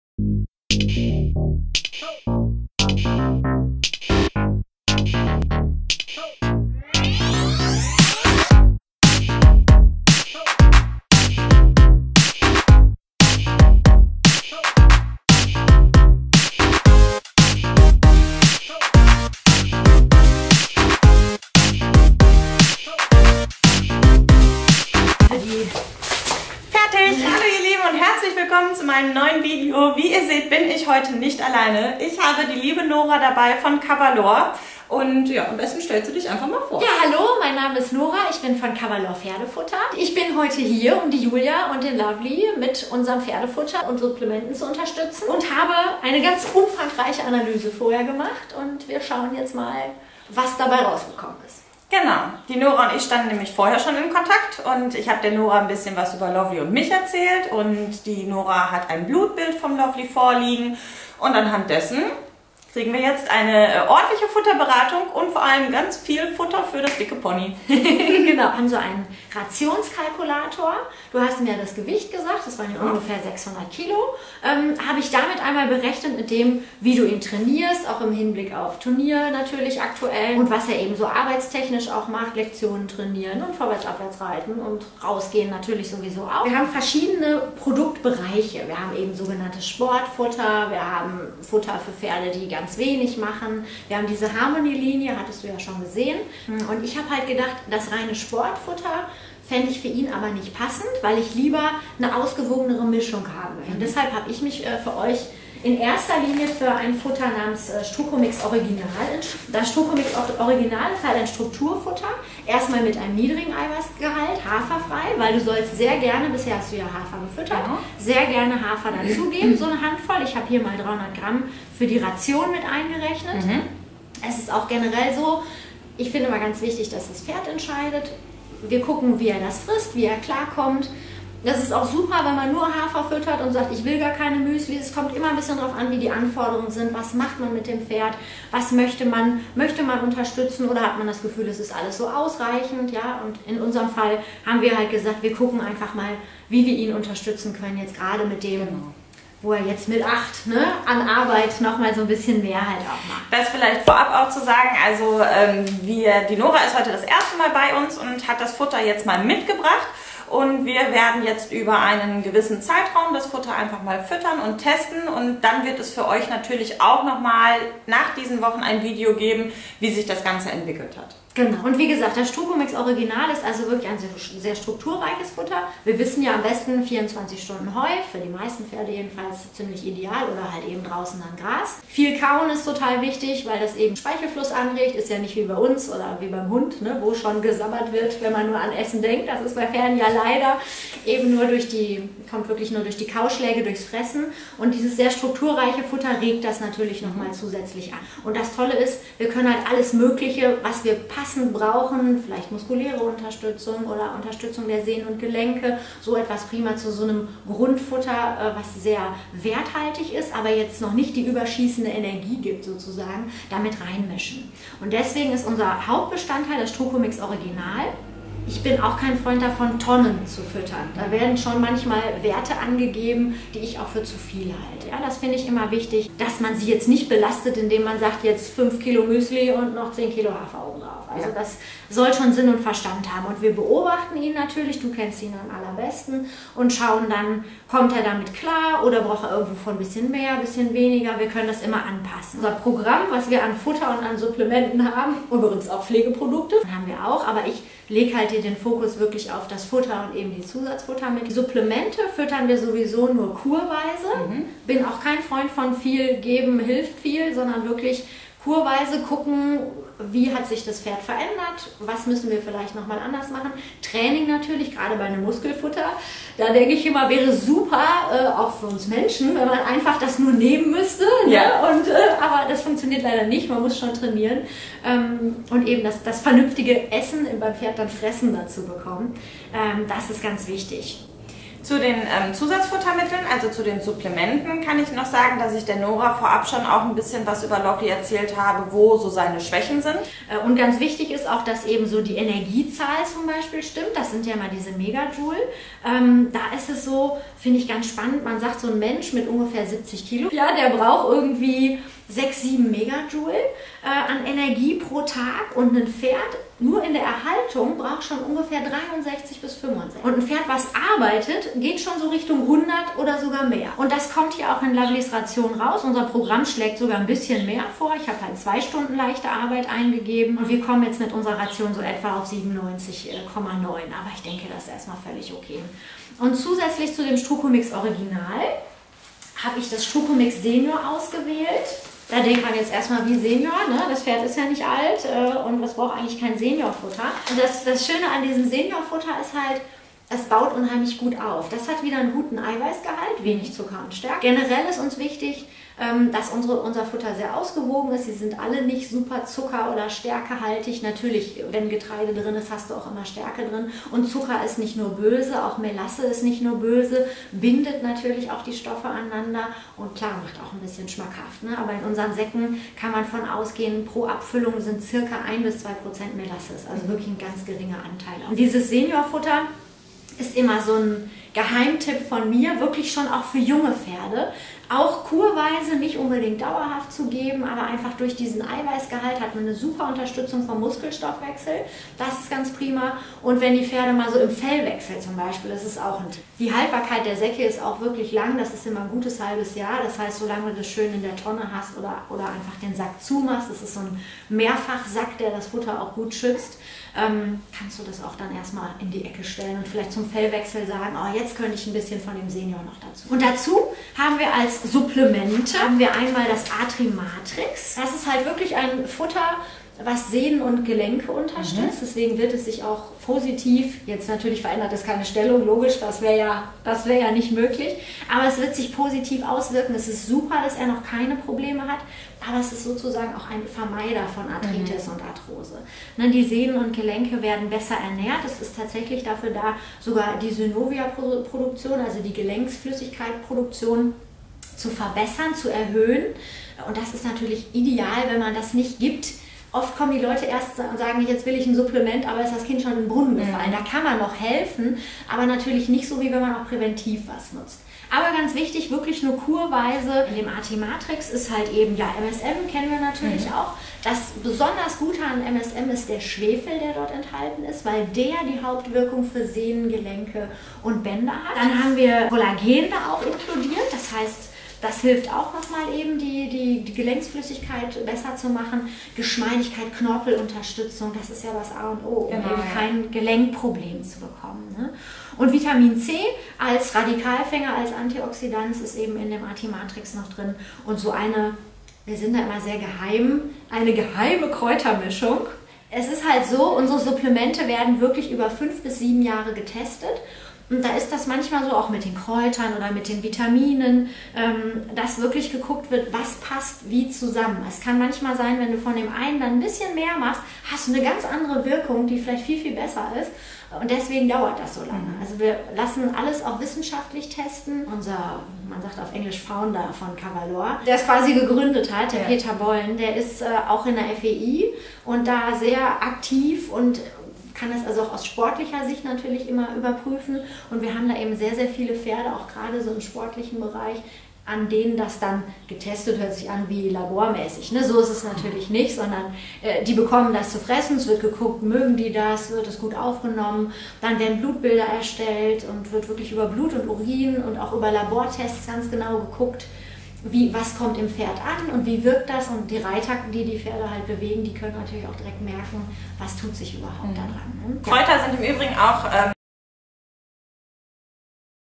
Julia Köster: Why you should feed your horse Cavalor
Julia Köster and her gelding „Deep Love“ – well-known in social media – are feeding CAVALOR and are reporting about it.
As follows here the first video with details about the feed and supplements as well as „Lovely’s“ feeding ration.
A lot of fun.